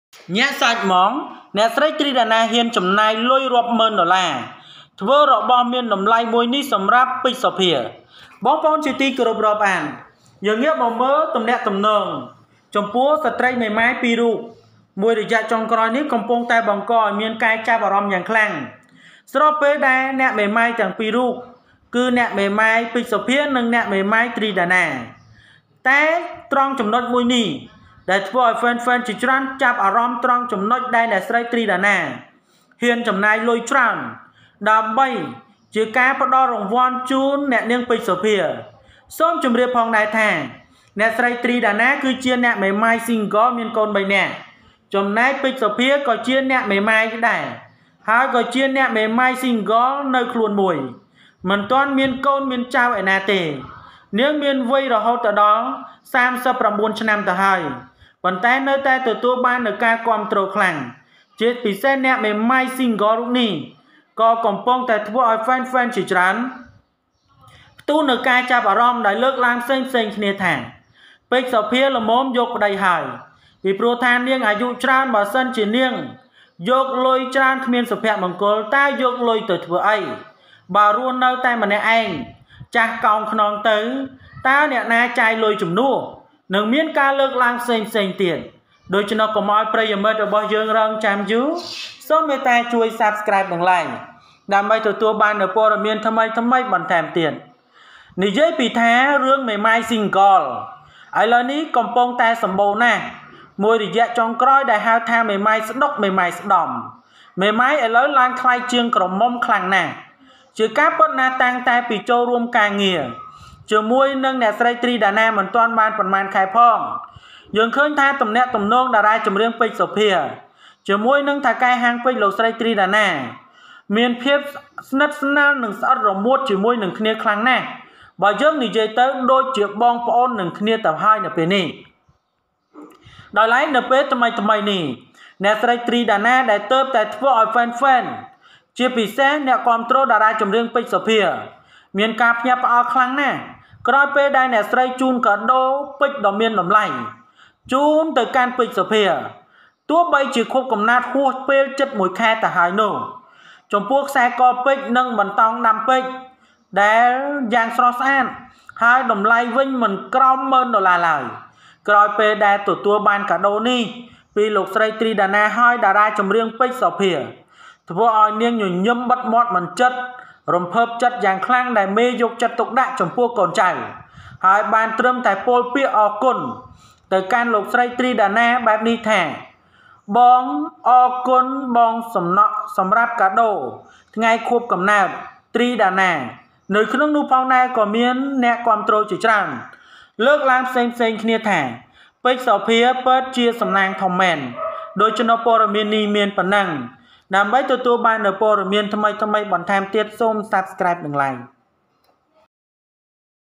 ញាក់សាច់ហ្មង ទ្រី ដាណា ហ៊ានចំណាយលុយរាប់មុឺនដុល្លារ ធ្វើមានតម្លៃមួយជូន ពេជ្រ សោភា ក្នុងថ្ងៃខួបកំណើត
ញាក់សាច់ហ្មង ទ្រី ដាណា ហ៊ានចំណាយលុយរាប់មុឺនដុល្លារ ធ្វើមានតម្លៃមួយជូន ពេជ្រ សោភា ក្នុងថ្ងៃខួបកំណើត Try Dana dared to spend tens of thousands of dollars to make a gift for Pich Sophea on her birthday.